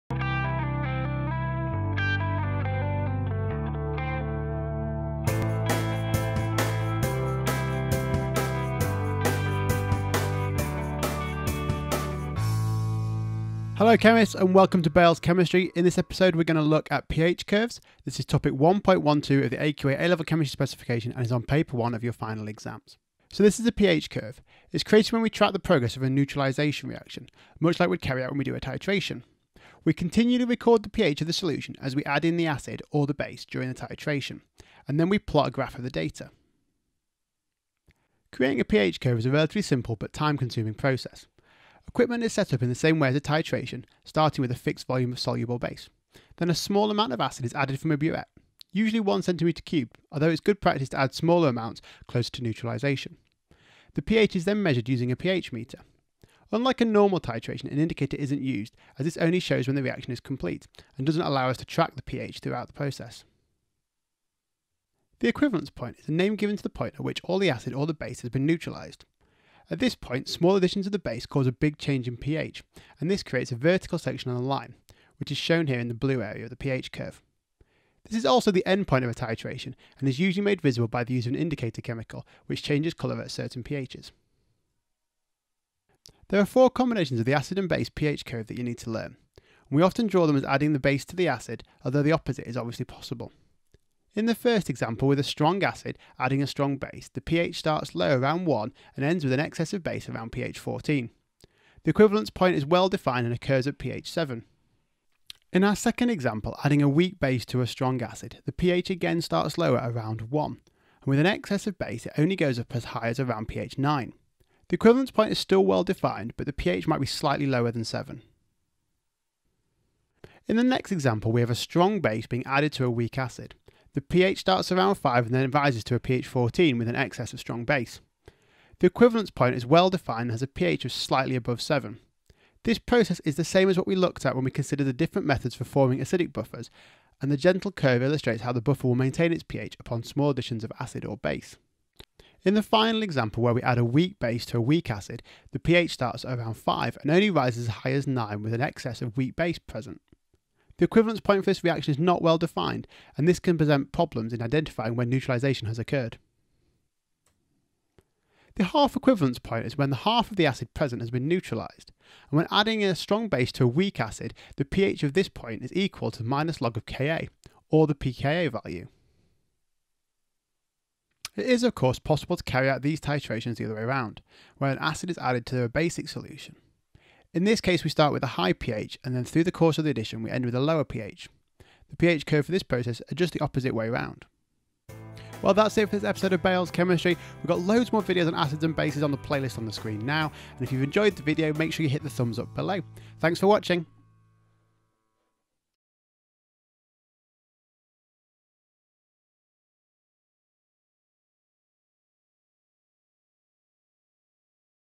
Drawing pH Curves, Paper 1 - AQA A Level Chemistry
In this video I talk through the 4 different combinations of pH curve you need to remember and some of their properties. This is AQA specification 1.12 Acids and Bases.